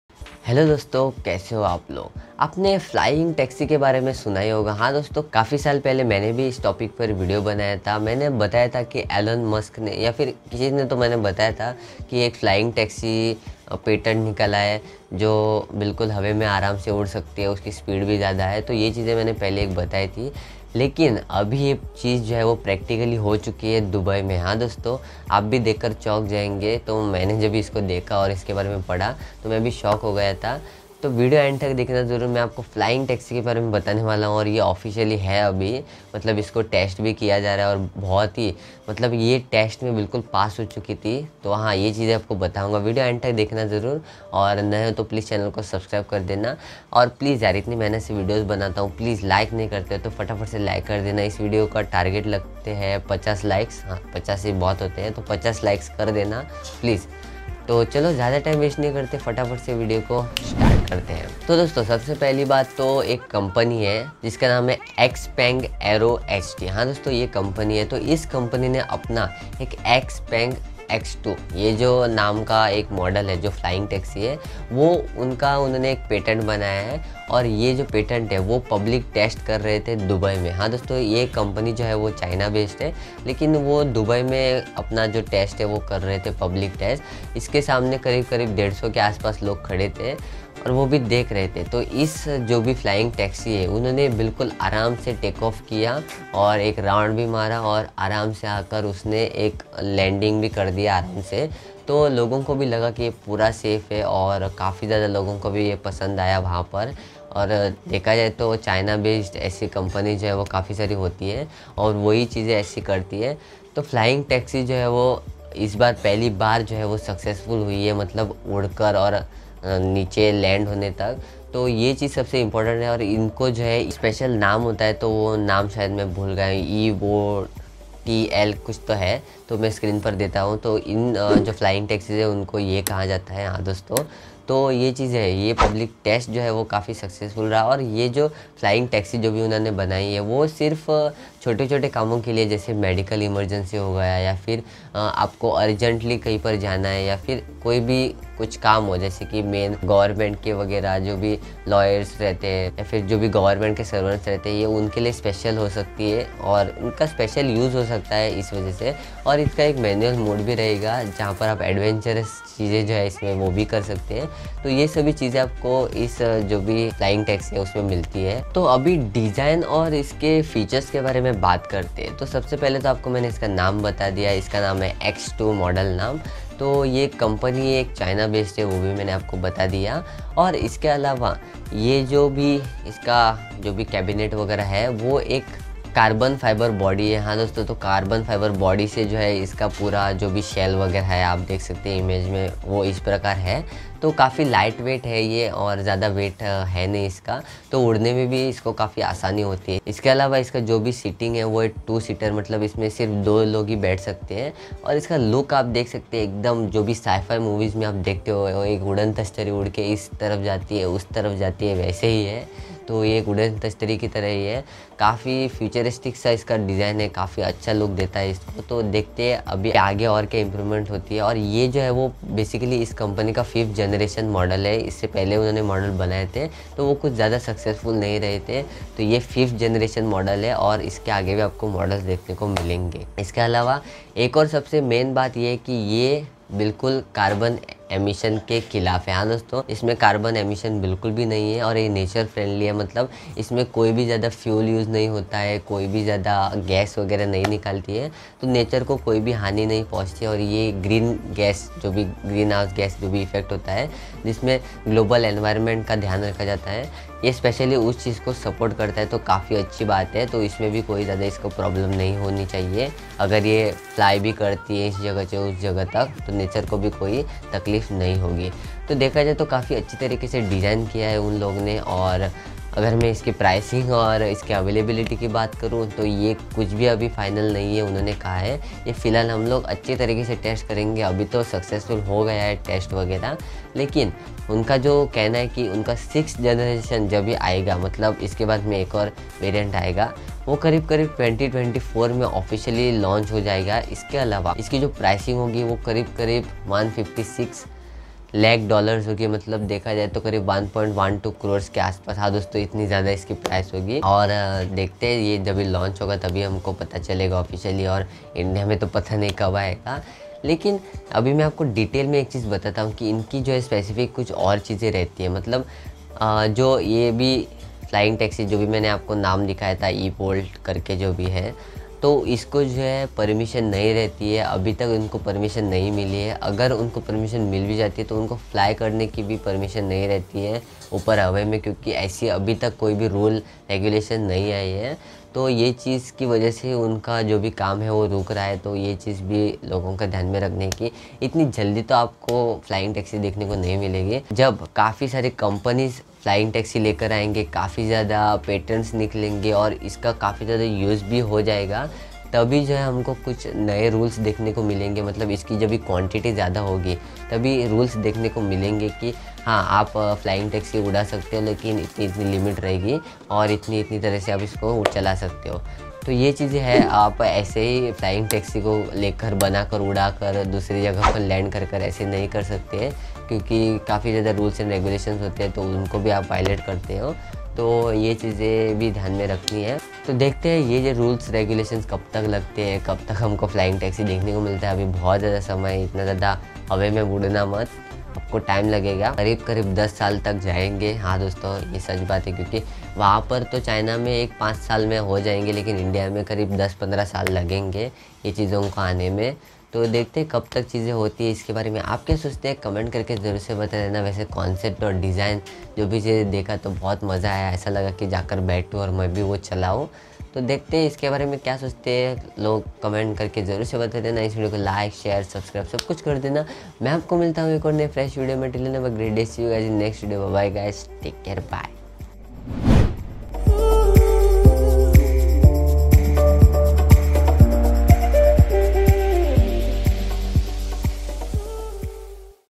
दुनिया की सबसे पहली Flying Car Aa Gayi Hai!
iss video main Maine bataya hai flying Taxi ki barein main jo abhi official ho chuki hai..

umeed karta hu ki apko video pasand aayega 🙏🏻😇